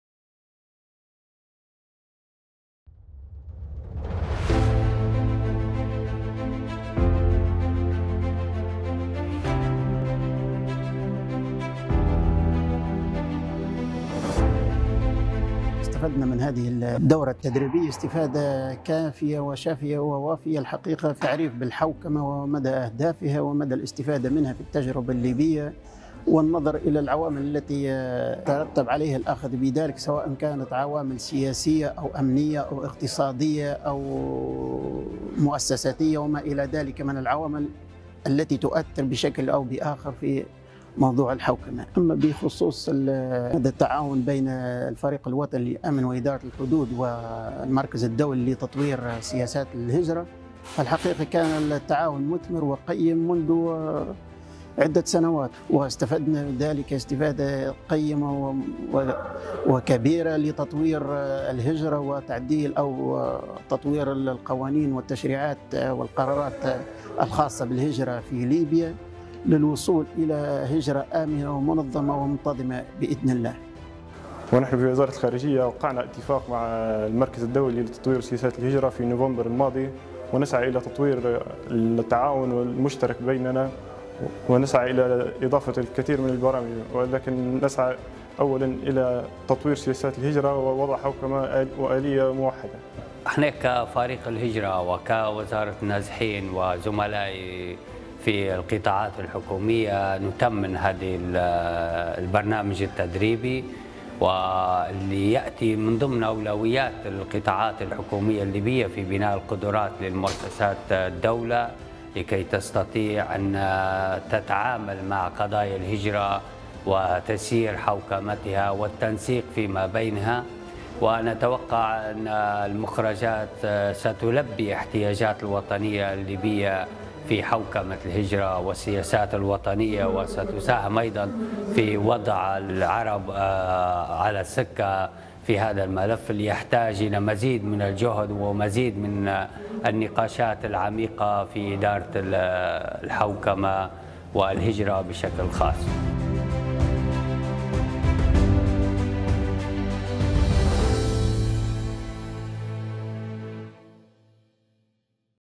ICMPD in Libya: Migration Governance Training Course with Libyan officials
In November 2020, within the framework of the “Strategic and Institutional Management of Migration in Libya” project, funded by the European Union, ICMPD launched a two-day Migration Governance Training Course for Libyan officials. Participants included civil servants at ministries involved in migration governance and staff of the Libyan House of Representatives. During the training, the participants discussed and shared their experiences on a wide range of theories, concepts, and institutional and legislative frameworks related to migration. The training, which was received with positive feedback, was conducted in partnership with the European University Institute’s (EUI) Migration Policy Centre (MPC), a leading research institute in the area of migration.